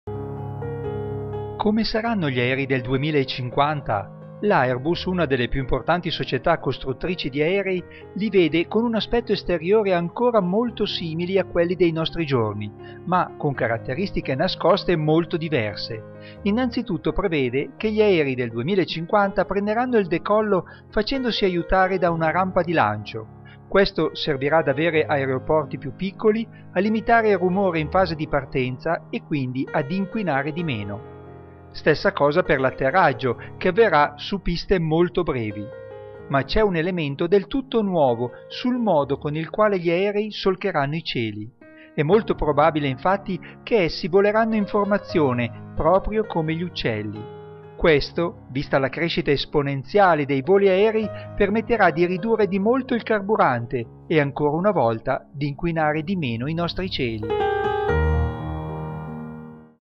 Aerei del 2050
Come voleranno gli aerei del futuro? Esteriormente sembreranno molto simili a quelli di oggi, ma in cielo voleranno come gli uccelli.